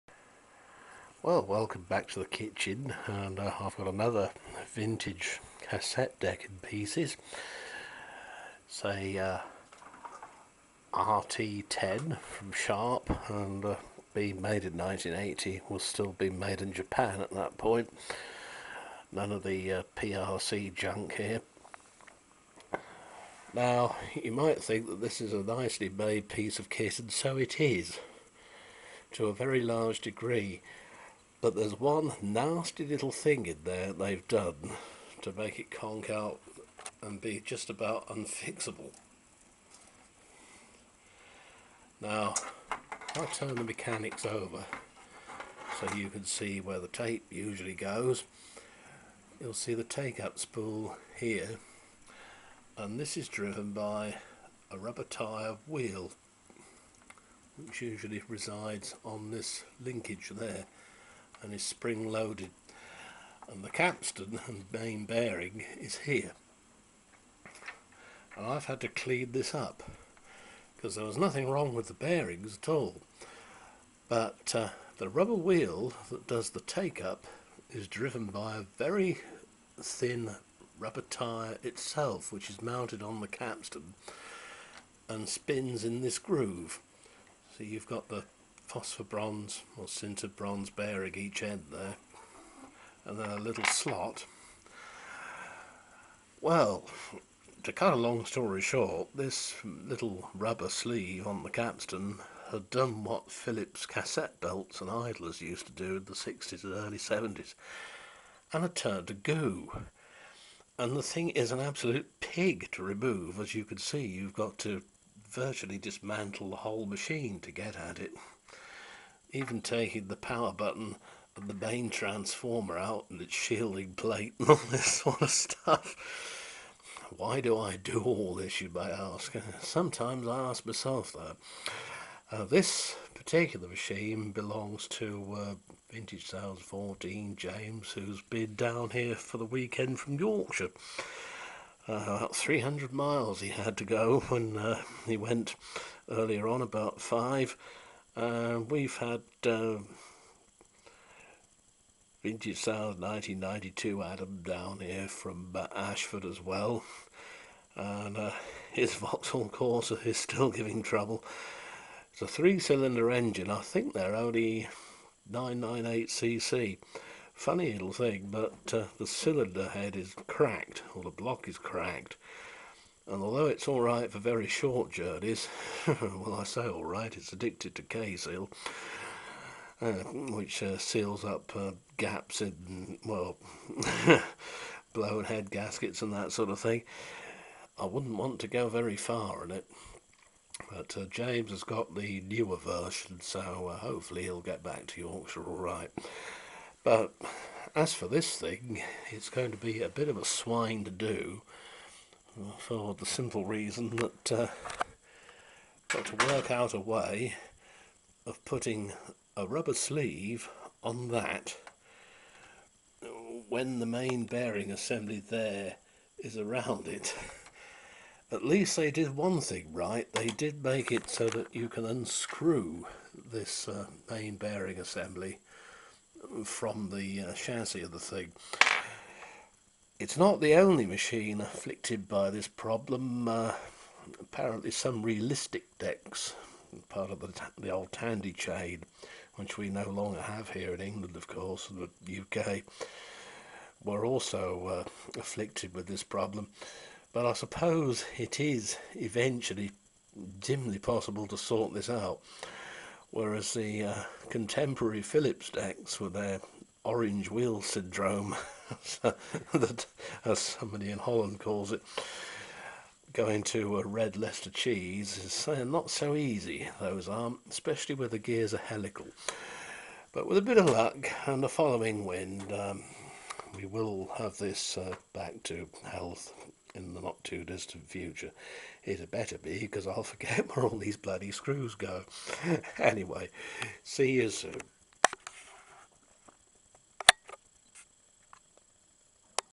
SHARP RT 10 cassette deck in trouble
A nice old thing, but spoilt by a rubber drive component that went to goo !